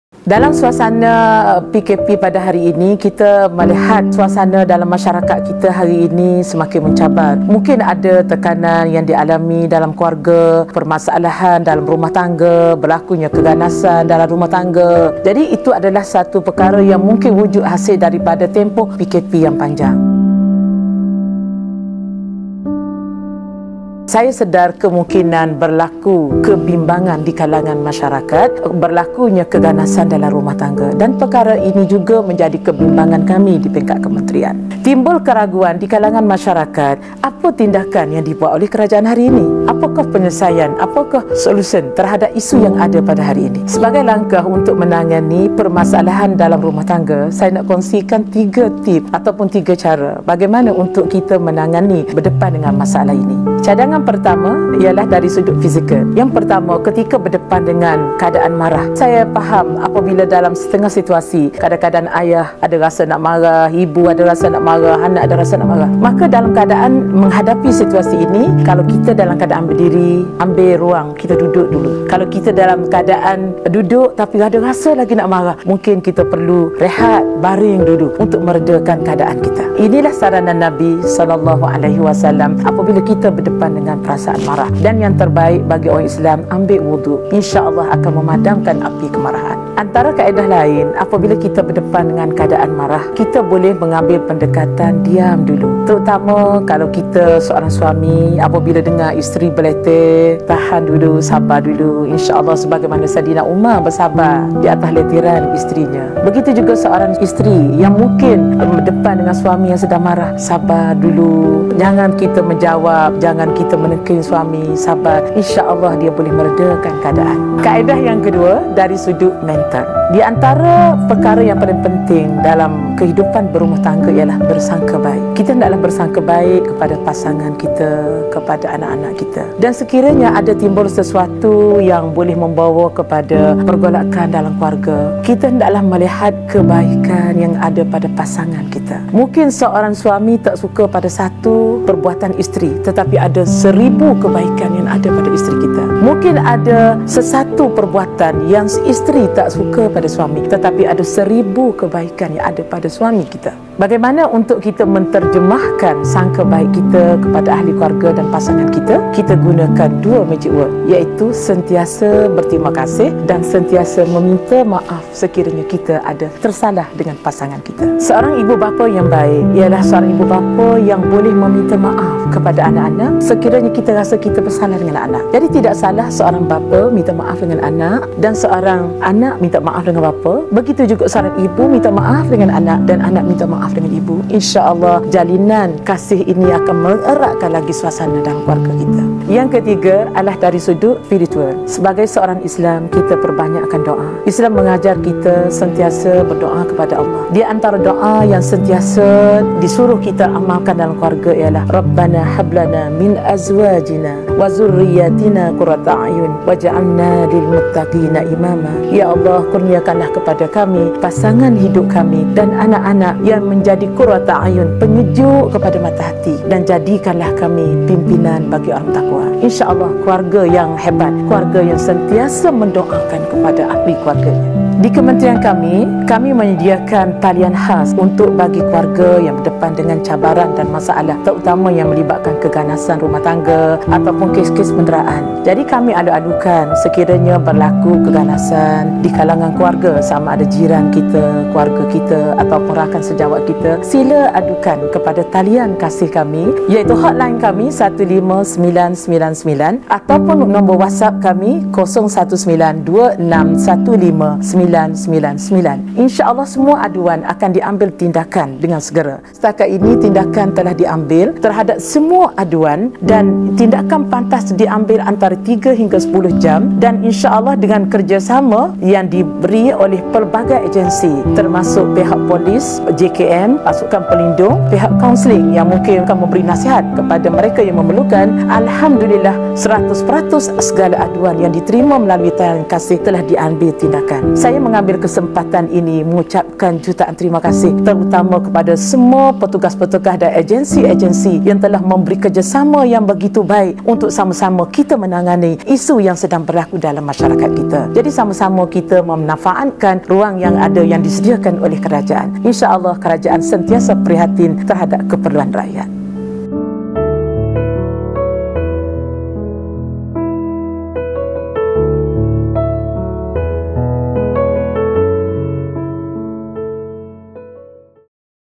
COVID 19: 3 Tips menghadapi PKP
Pesanan oleh YB. Dato' Siti Zailah Mohd Yusoff, Timbalan Menteri Wanita, Keluarga dan Masyarakat.